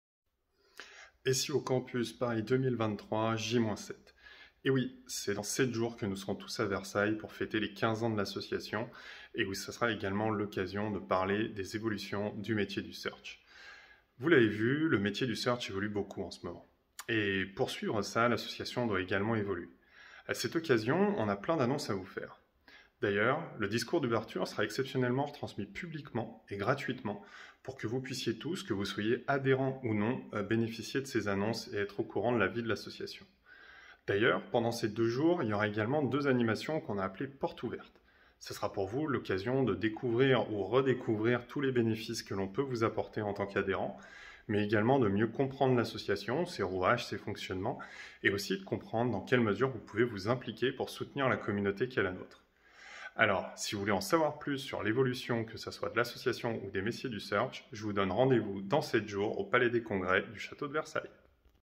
J-7 avant le SEO CAMP'us Paris 2023
Le SEO CAMP'us Paris 2023 a lieu dans une semaine ne ratez pas cet événement emblématique de l'association !

Patrick Valibus, président du SEO CAMP, vous parle de l'événement.